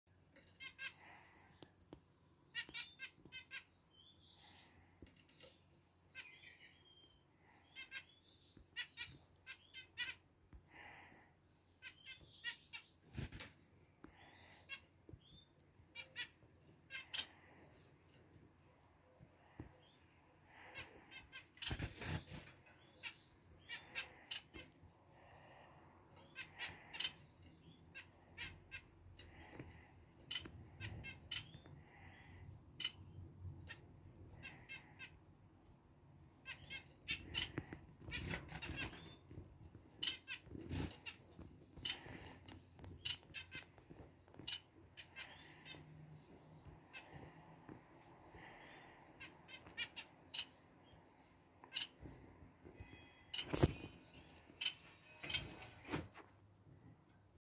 Moment opname volière__2e en 3e jaars__En jonge van een aantal weken.
De vogels bleven zo stil zitten..Dat ze onder hypnose waren :)
Some birds